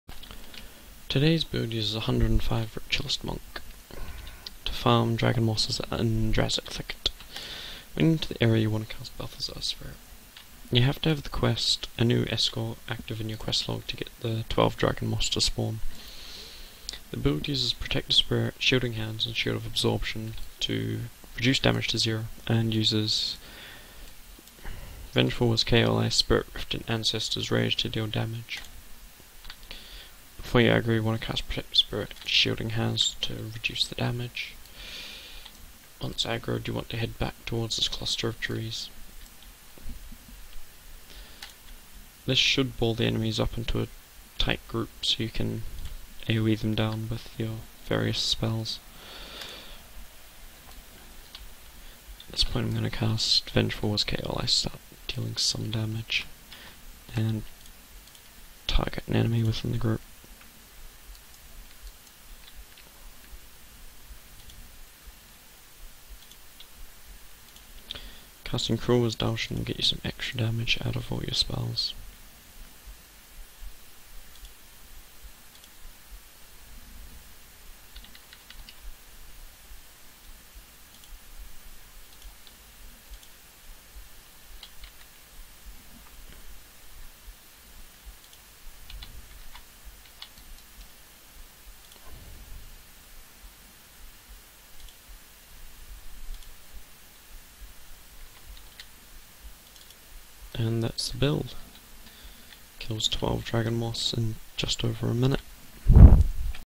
Rt/Mo Dragon Moss HM Farmer
This build uses a Ritualist/Monk to farm 12 Dragon Moss in HM in hopes of getting gold items / rare crafting materials.

Skills:
Vengeful Was Khanhei, Spirit Rift, Ancestor's Rage, Protective Spirit, Shielding Hands, Shield of Absorption, Cruel Was Daoshen, Balthazar's Spirit.

Attributes:
15 Restoration Magic
13 Channeling Magic
10 Protection Prayers

Equipment:
5x Herald's Insignia
3x Rune of Superior Spawning Power
1x Rune of Superior Restoration Magic
1x Rune of Superior Channeling Magic
Totem Axe
Droknar's Restoration Focus